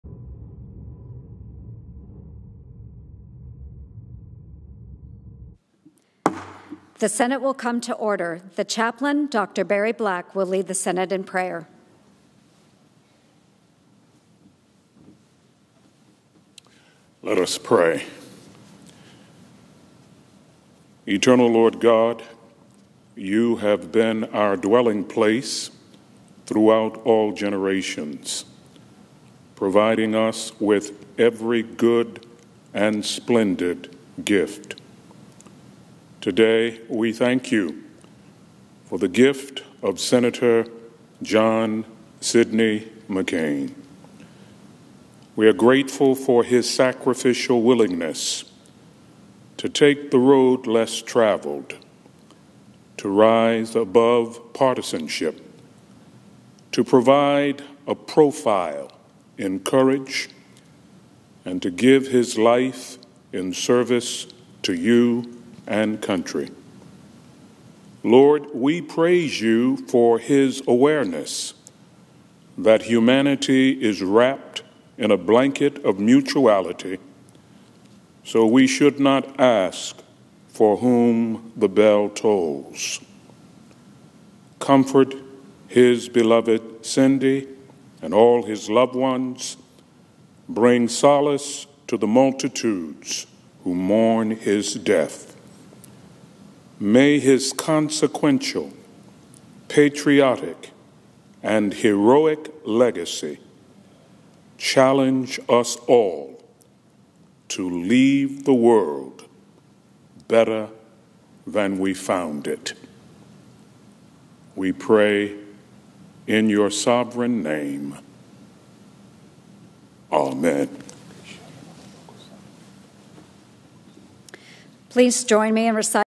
Daily Congress Prayer CLASSIC Aug. 27, 2018 -- McCain
The Chaplain, Dr. Barry C. Black, offered the following prayer: Let us pray. Eternal Lord God, You have been our dwelling place
throughout all generations, providing us with every good and splendid gift. Today, we thank You for the gift of Senator JOHN SIDNEY MCCAIN. We are grateful for his sacrificial willingness to take the road less traveled, to rise above partisanship, to provide a profile in courage, and to give his life in service to You and country. Lord, we praise You for his awareness that humanity is wrapped in a blanket of mutuality, so we should not ask for whom the bell tolls. Comfort his beloved Cindy and all his loved ones. Bring solace to the multitudes who mourn his death. May his consequential, patriotic, and heroic legacy challenge us all to leave the world better than we found it. We pray in Your sovereign Name. Amen.